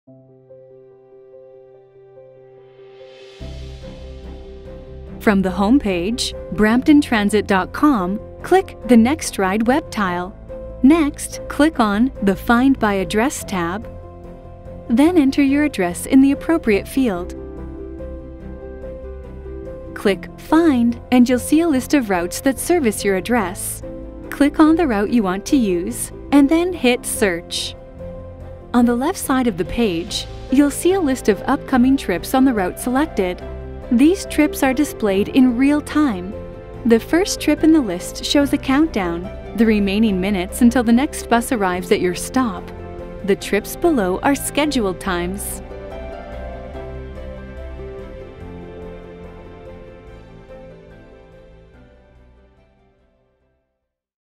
Next Ride | Find By Address
Did you know you can search for your next bus – in real-time – using your address?